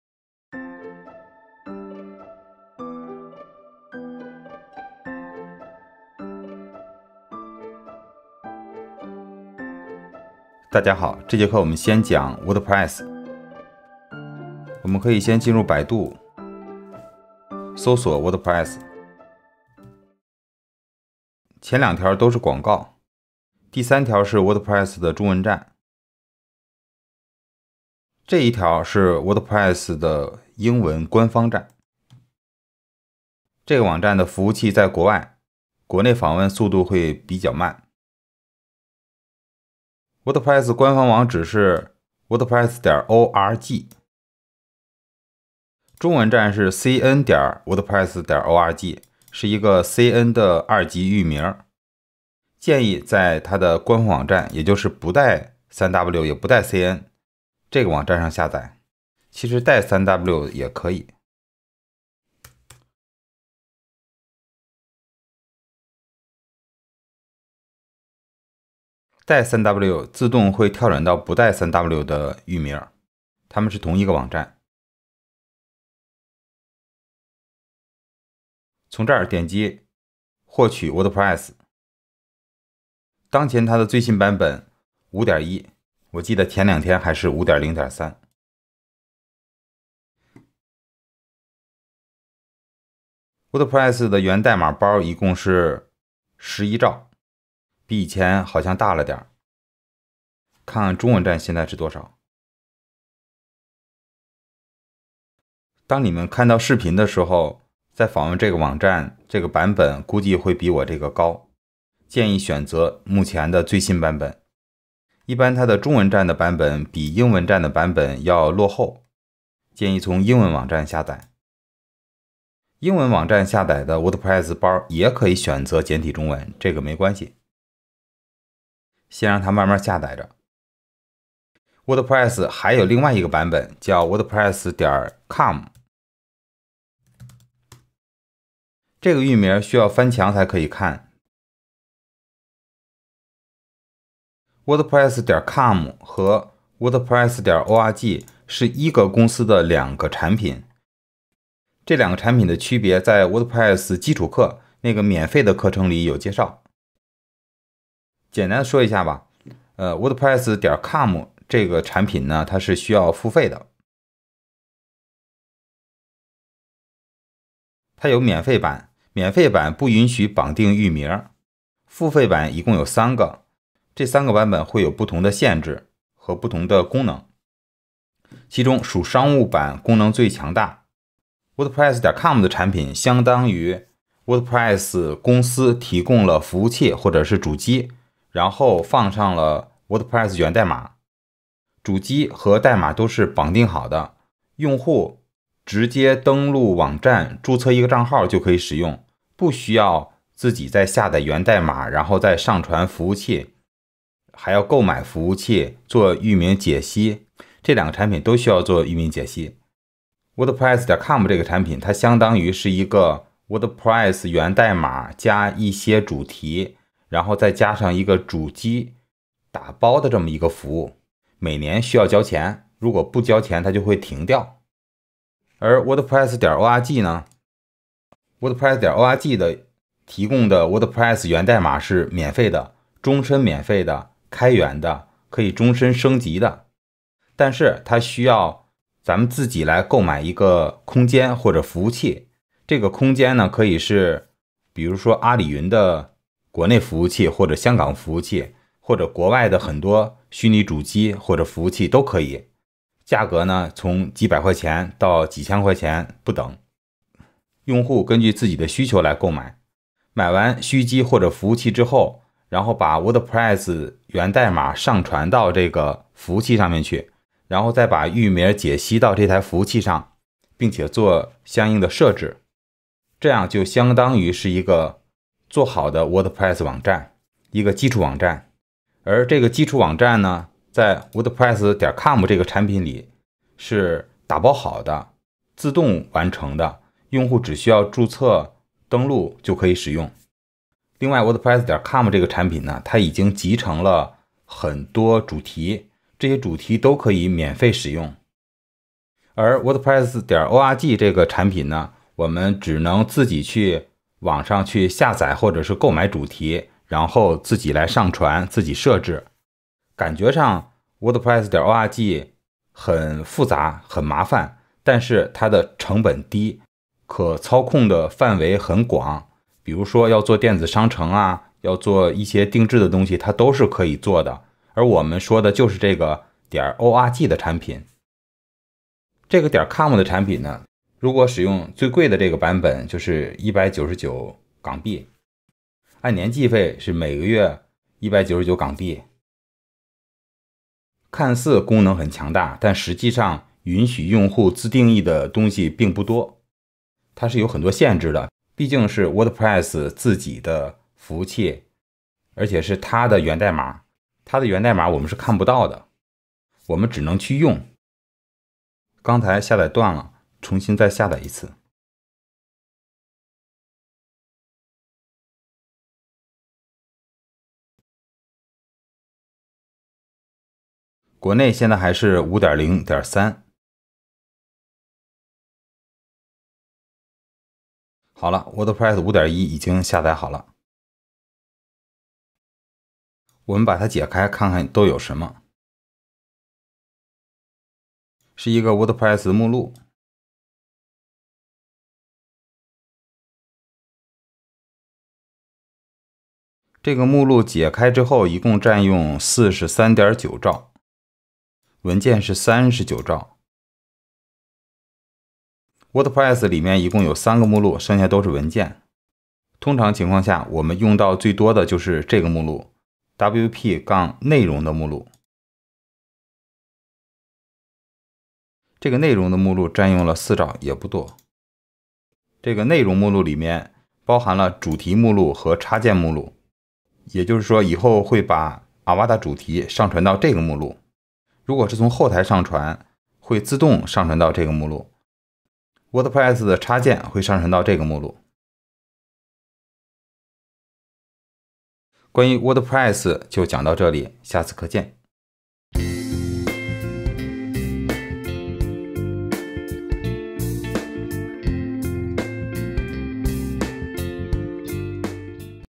获取WordPress以及org与com的区别
大家好，非常感谢 选择achair制作的Avada主题教程。相信有很多同学多少了解过WordPress，看过WordPress主题做的网站，比如Avada主题，这种方式做网站，不需要编程经验，不需要修改代码，可以做出像主题的英文演示站，那种非常漂亮、国际范的响应式网站，而且这种网站建设的效率很高，成本低，以后的网站维护方便。